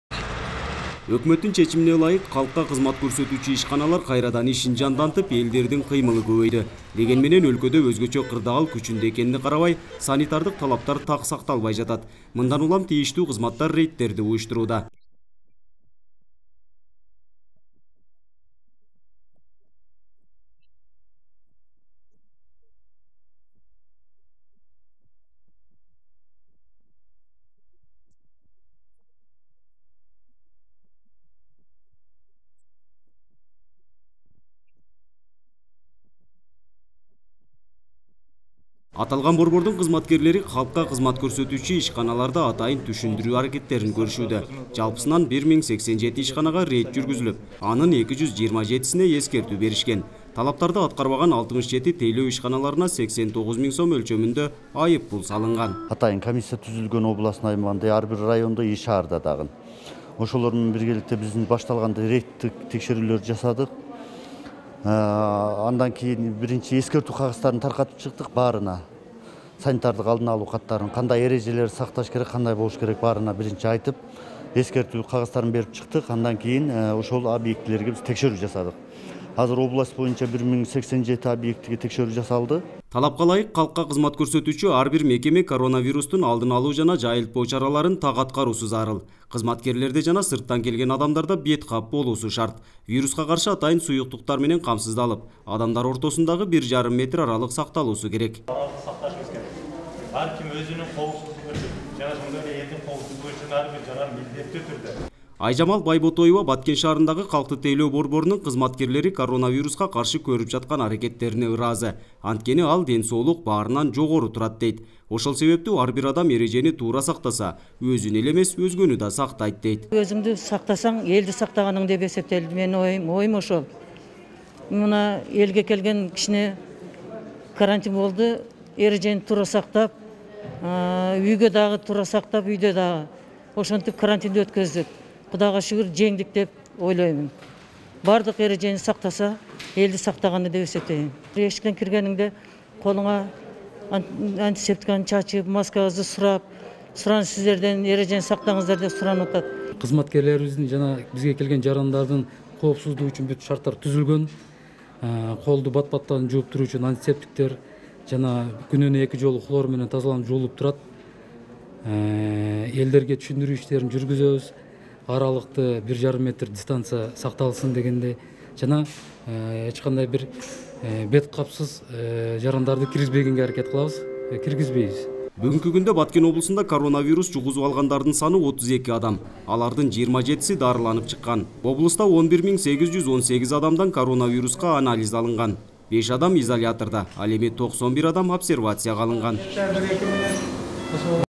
Маска такпагандар көбөйдү
Өлкөдөгү кырдаалга байланыштуу Баткен облусундагы калкка кызмат көрсөтүүчү мекеме ишканалардын санитардык абалы текшерилип турат. Атайын түзүлгөн комиссия 1087 ишканага рейд жүргүзүп, анын 227сине эскертүү беришкен. Талаптарды аткарбаган 67 тейлөө ишканаларына 89 миң сом өлчөмүндө айып пул салынган. Санитардык абалды көзөмөлдөө дагы деле уланууда.